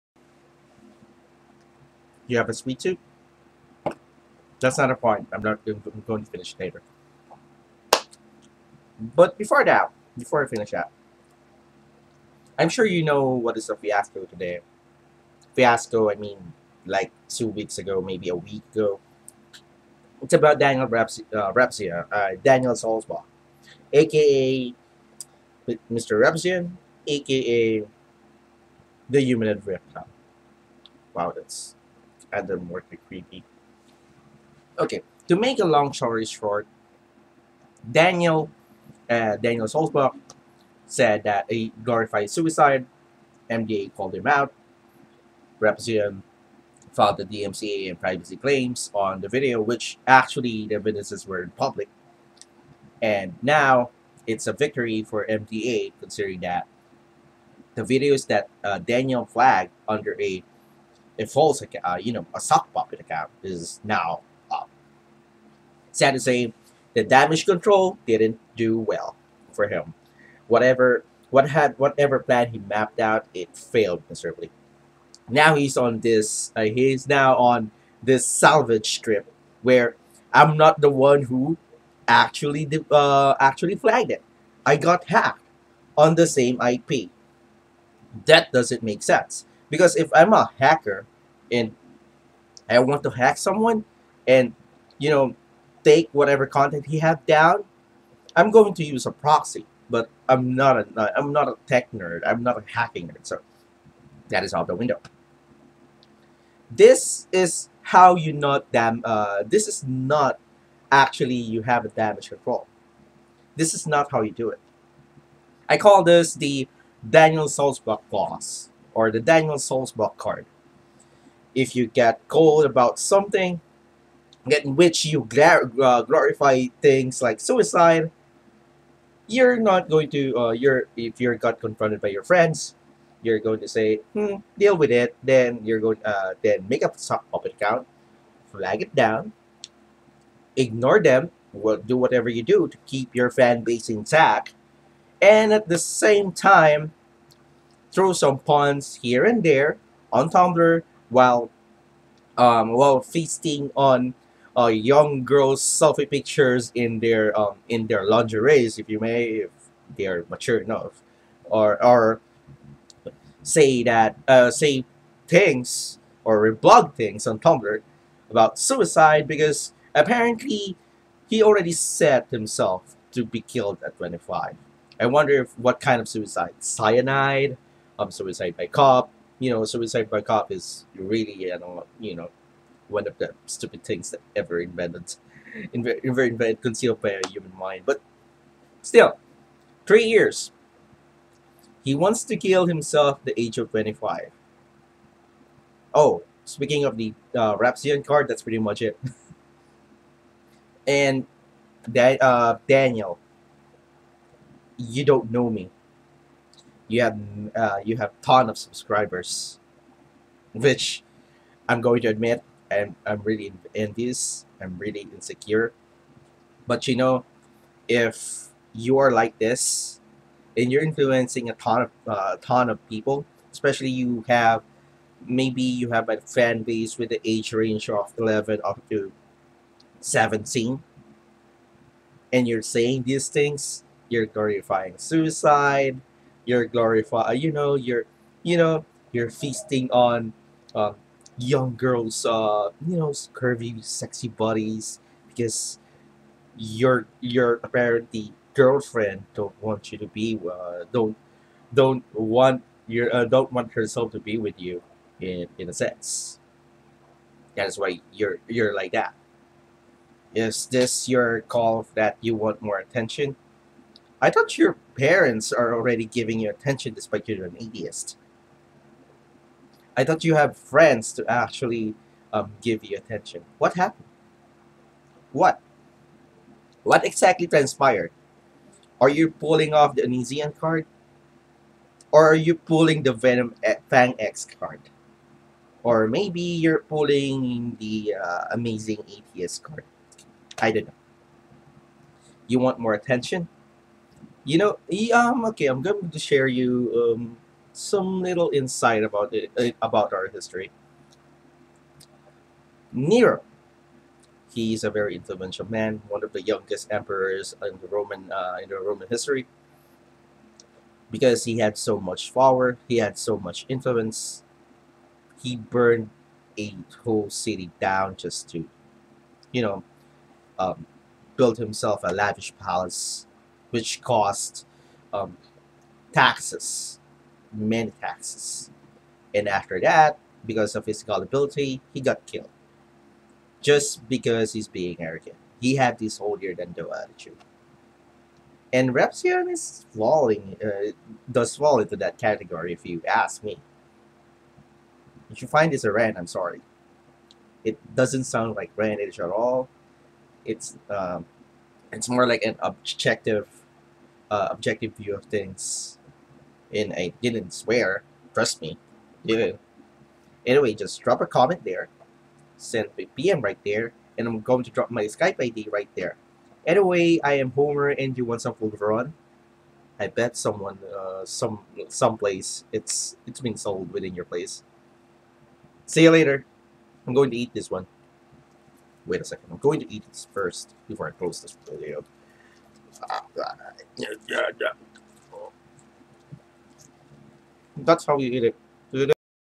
This is NOT how you damage control - MrRepzion style.
There is no chance in hell that someone can do a damage control on this one. MrRepzion, have some humility and admit defeat. That's all been asked. Running away is not helping yourself.

Daniel, you're 22 years old. Grow some balls and man the hell up.

Skype ID: ramzarugliabeoulve